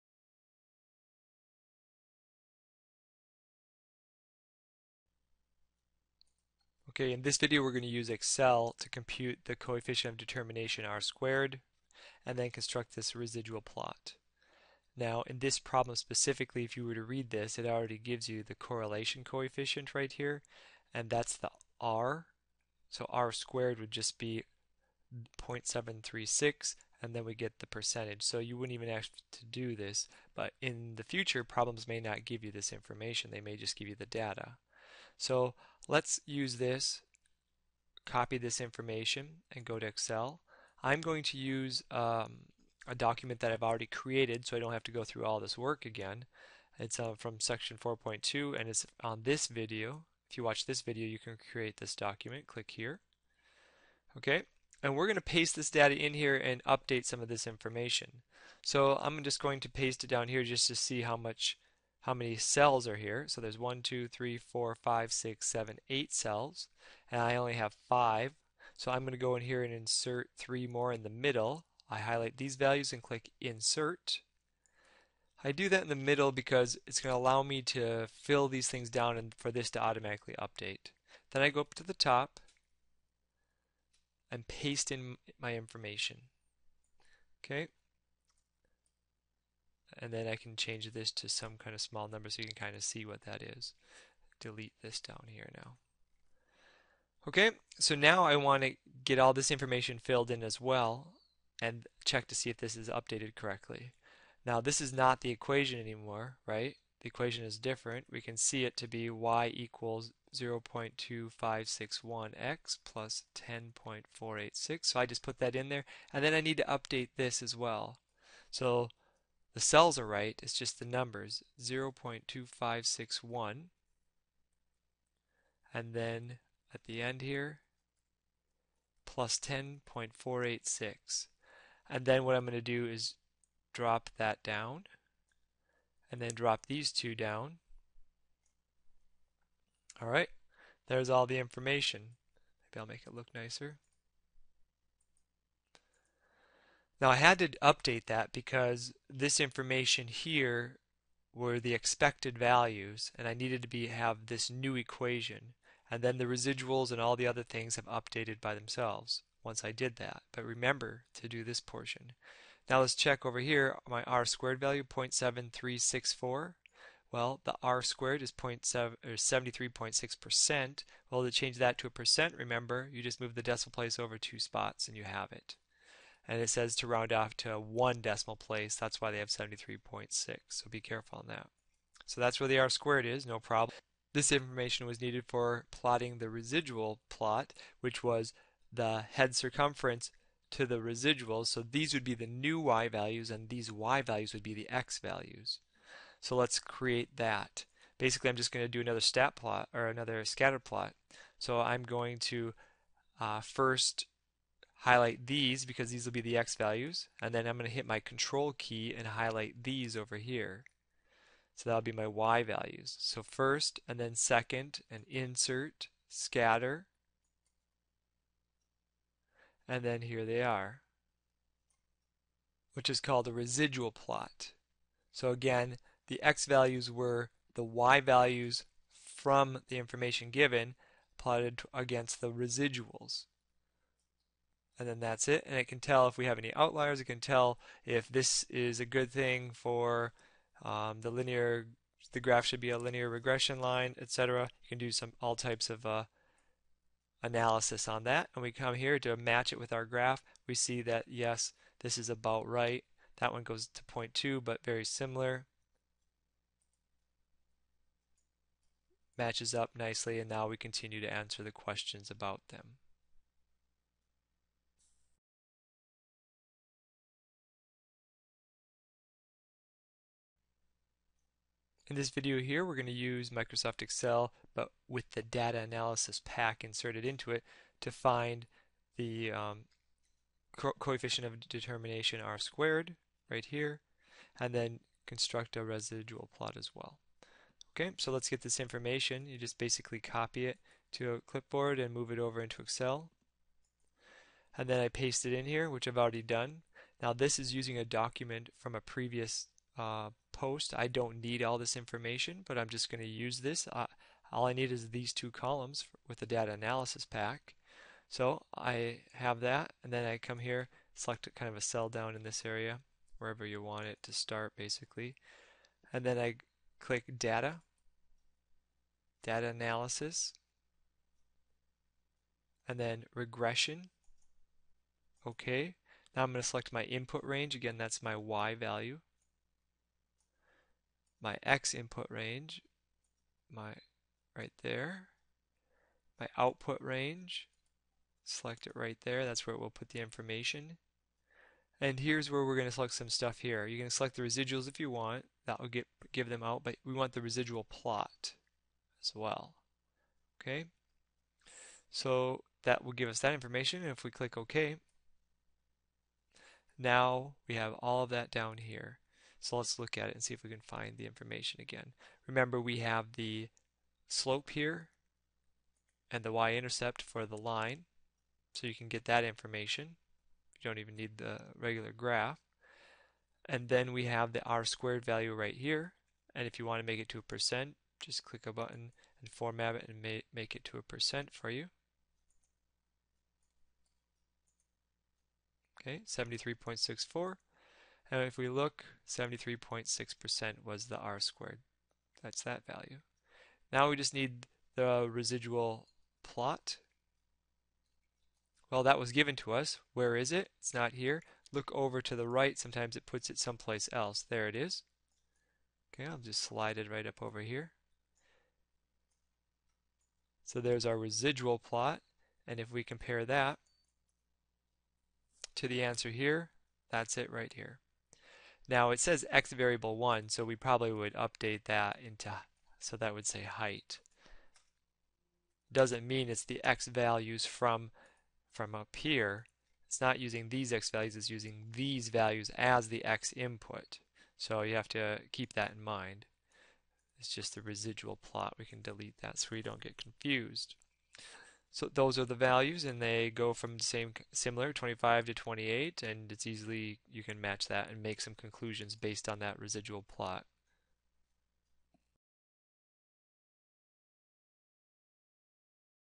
Excel R squared and residual plots - SEE DESCRIPTION FOR ERROR
ERROR IN VIDEO - AT 3:26 I should highlight x values.  Make note. I can't fix this with a note  - youtube has removed that feature.

This video shows how to figure out R Squared (coefficient of determination) and then Residual Plots.  I do this first without the data analysis pack, and then I show how to do it with the data analysis pack.